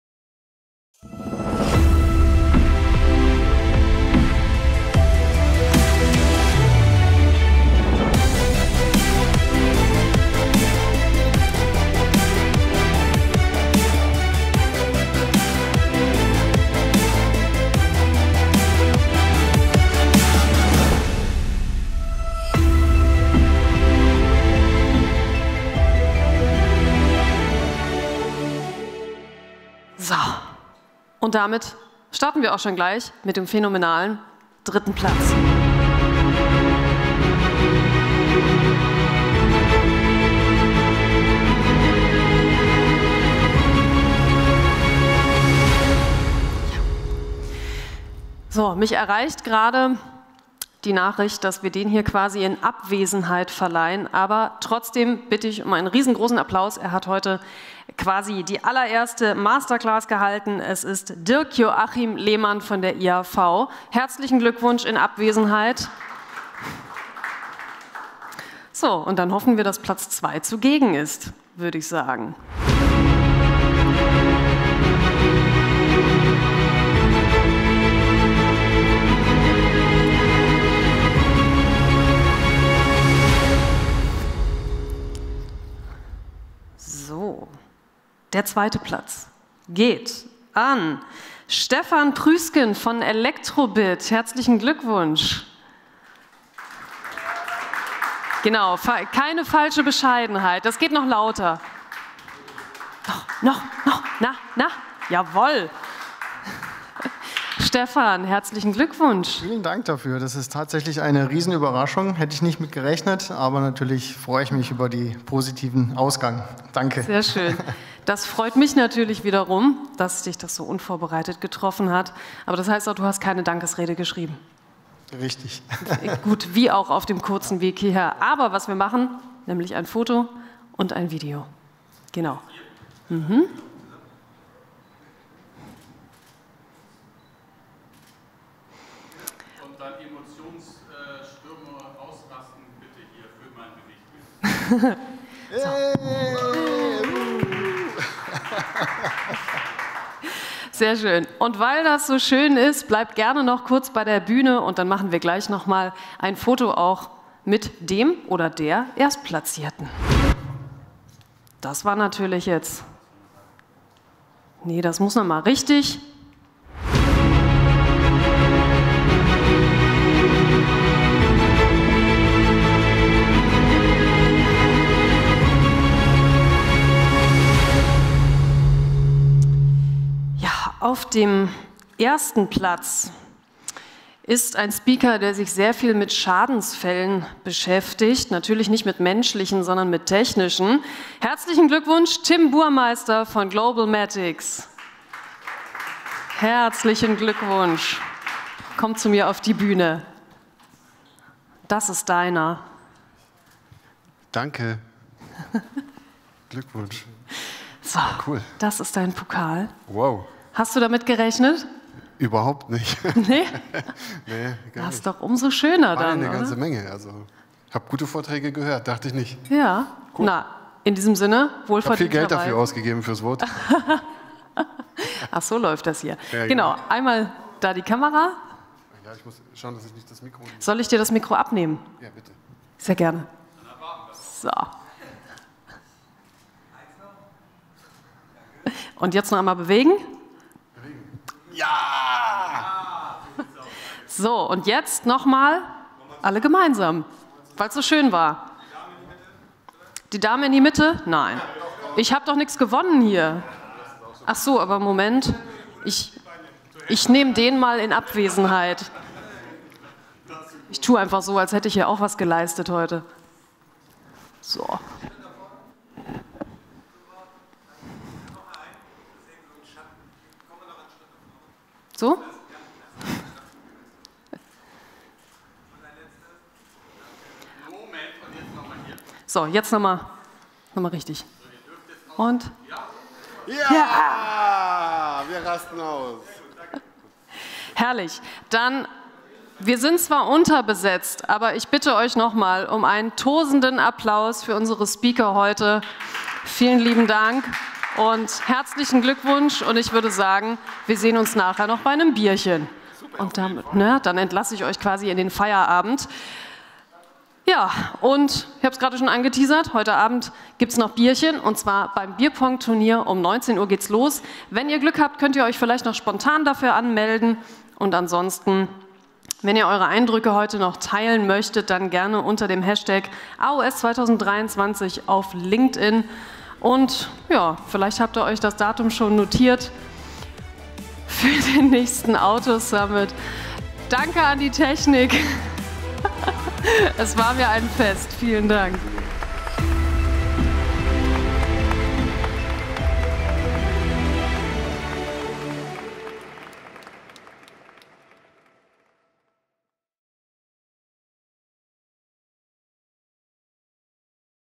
Verleihung der Auto Summit Speaker Awards Masterclasses Tag 1 2023
GEWINNER:
1. Platz Timm Burmeister (Globalmatix)
2. Platz Stefan Pruisken (Elektrobit)
3. Platz Dirk Joachim Lehmann (IAV)

MODERATION: Inken März